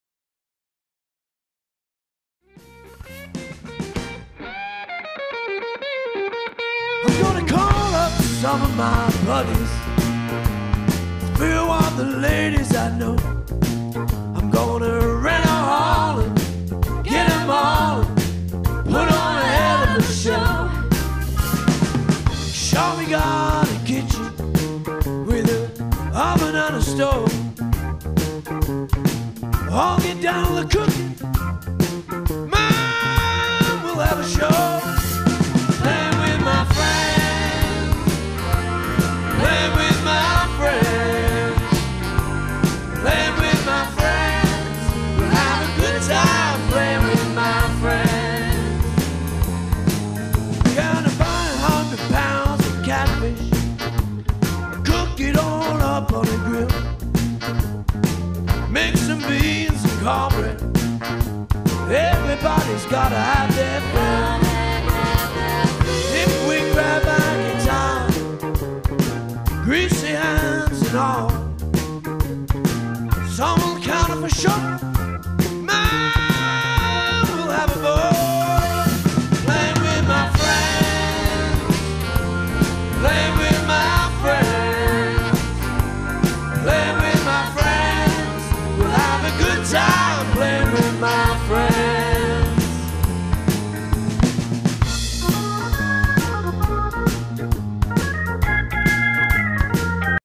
Jigsaw Community Festivals Jinney Ring Blues Festival 2011
Erecting and dismantling a marquee and stage at the Jinney Ring Blues Festival